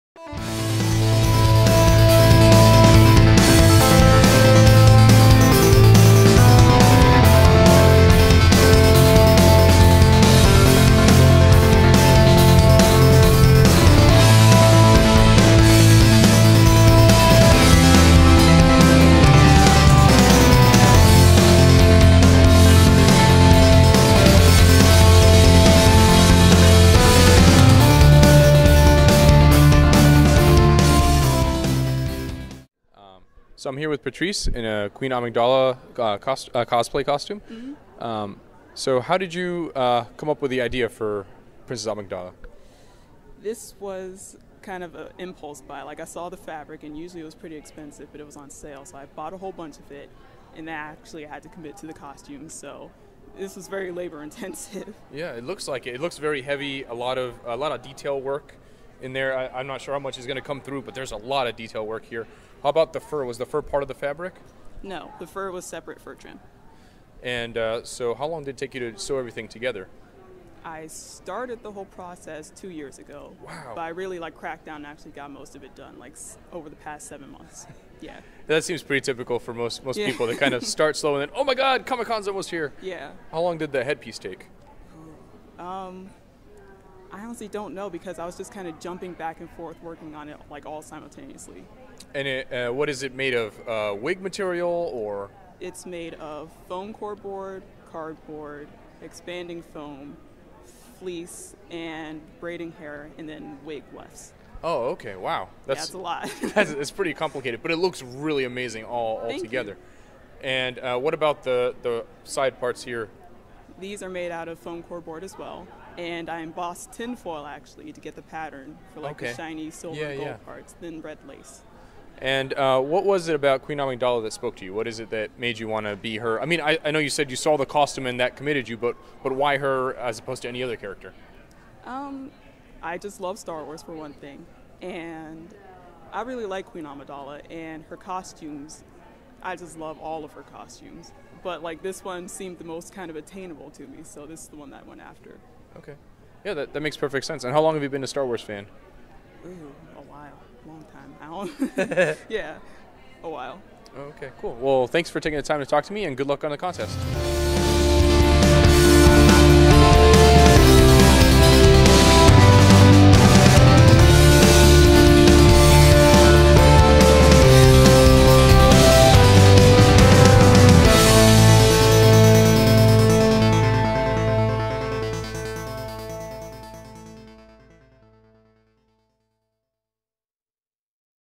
Baltimore Comic-Con 2018: Cosplay Interview with Patrice
An interview with Patrice about her very elaborate Queen Amidala cosplay costume.

More Baltimore Comic-Con 2018 coverage, including audio-only interviews with creators like Tom King,